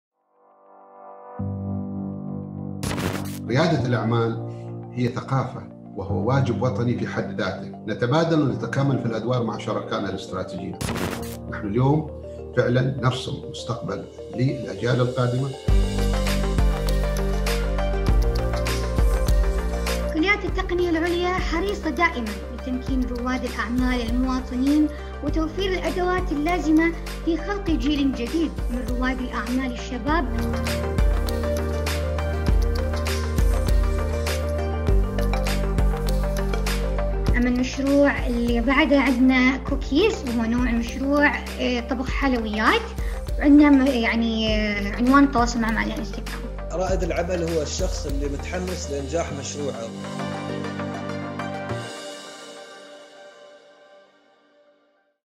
“Montajati” Entrepreneurship Bazaar
‏Over 2 consecutive days, the Entrepreneurs Bazaar “Montajati'” strives to consolidate the culture of entrepreneurship through supporting small and medium enterprises. 11 significant projects were presented by students of the Higher College of Technology- Fujairah. Details in this video!